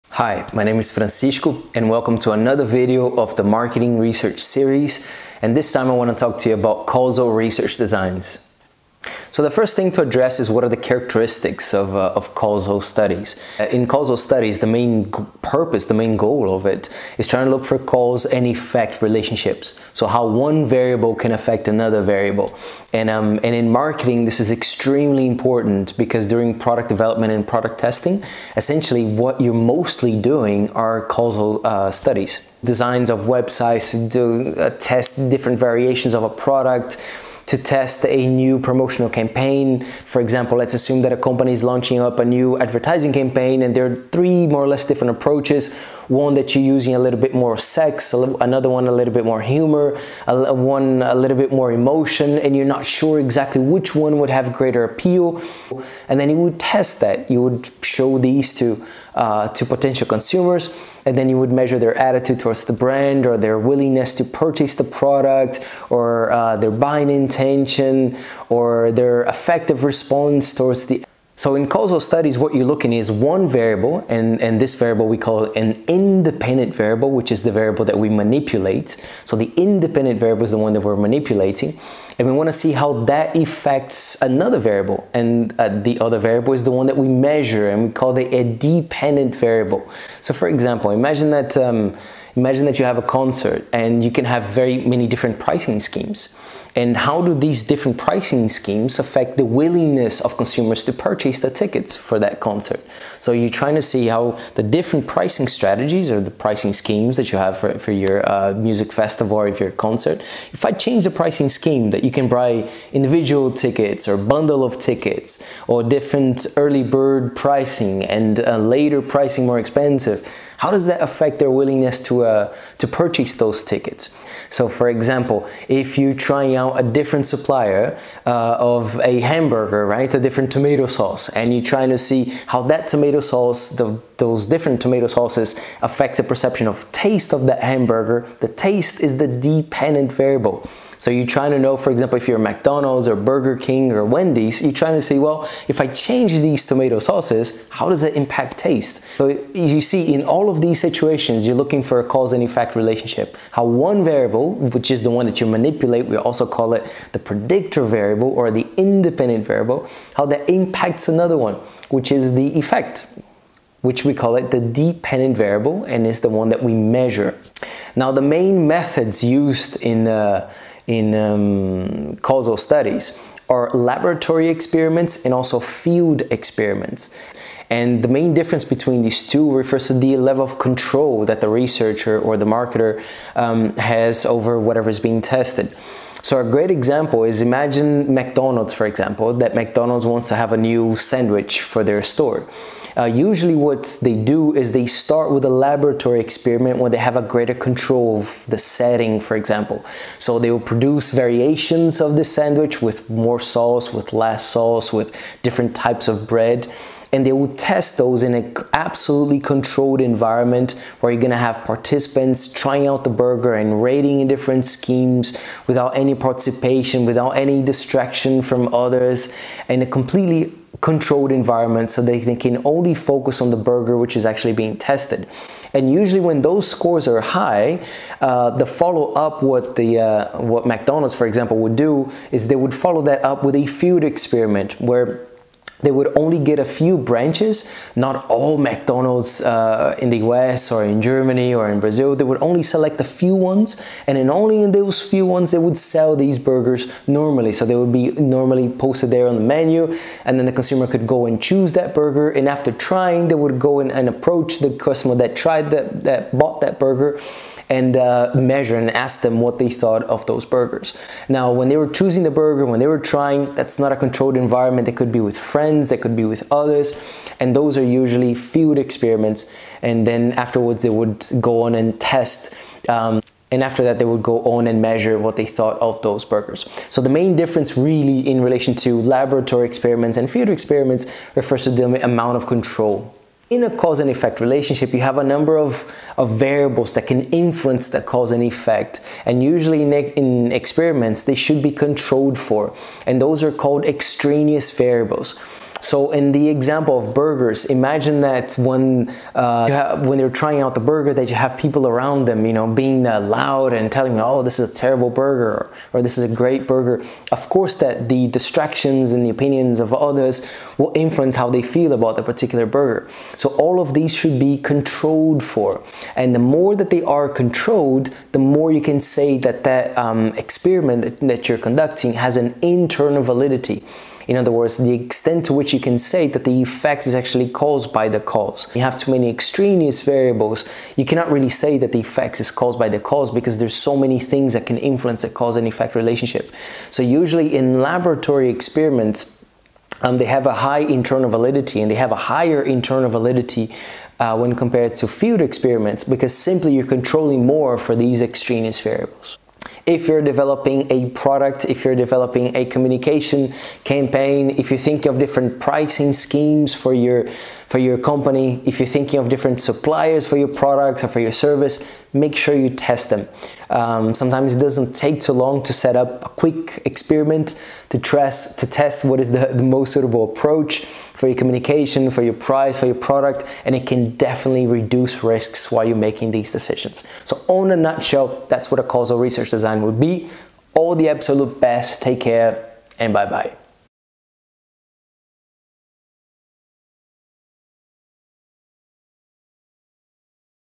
Causal Research Design / Marketing Research #7
In the seventh video of the "Marketing Research" series, Francisco Tigre Moura discusses causal research designs, methods and applications in marketing.